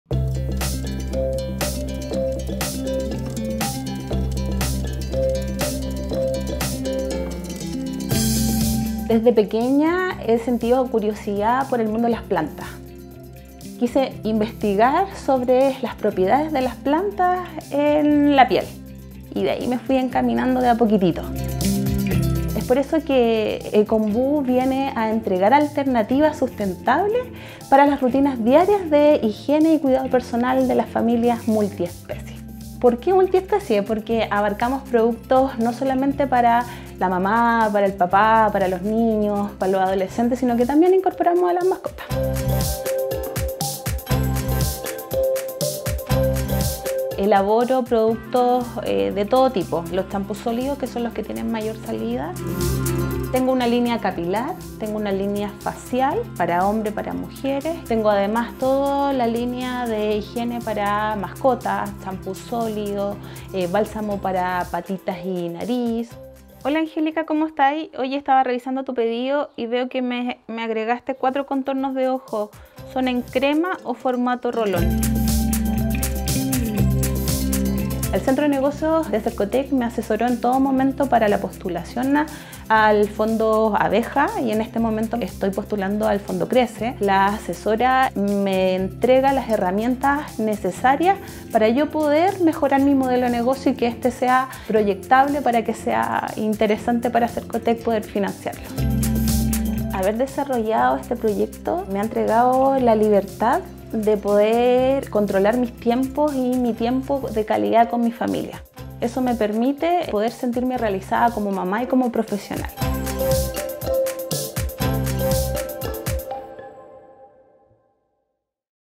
CasosdeÉxito | Ecoombú
Casos de éxito Centro de Desarrollo de Negocios Rancagua.
Emprendedora: Loreto Orellana
Emprendimiento: Ecoombú